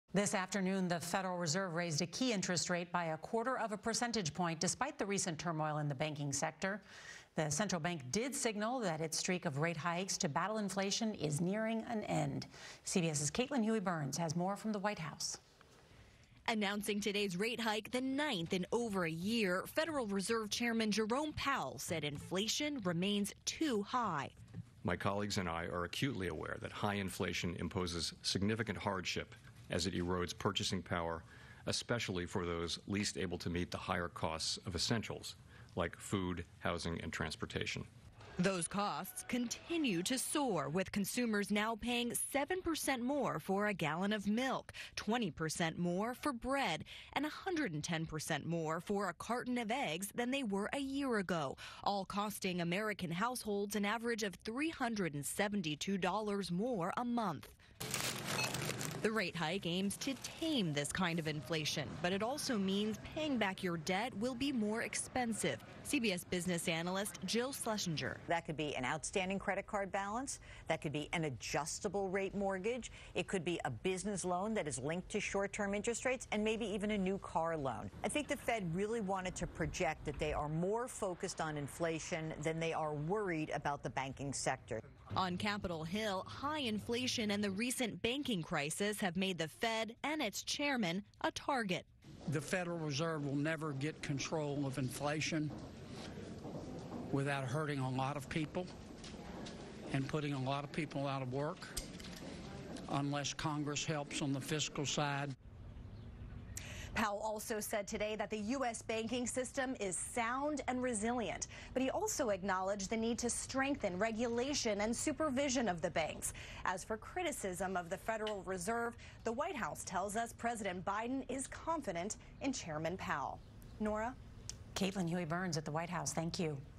Federal Reserve raises interest rates despite recent banking troubles
The Federal Reserve raised interest rates by a quarter of a percentage point, despite several recent bank failures. The Fed signaled that the wave of rate hikes may be nearing an end. Caitlin Huey-Burns takes a closer look.